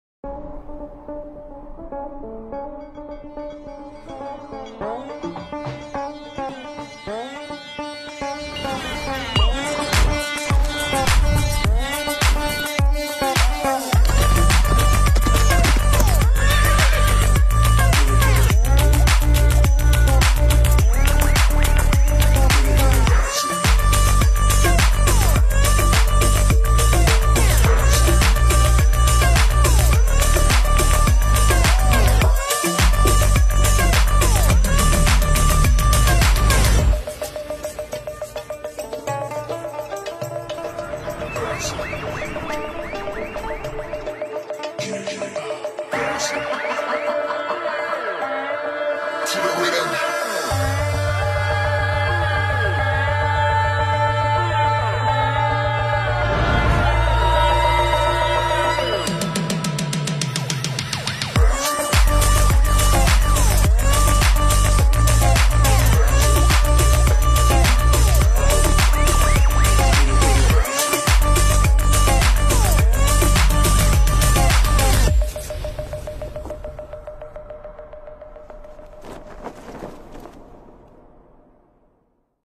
فيديو ترويجي للمغرب يُعرض في هذه الفترة في عديد القنوات الفضائية العالمية Visit Morocco
جديد..فيديو ترويجي للمغرب يُعرض في هذه الفترة في عديد القنوات الفضائية العالمية .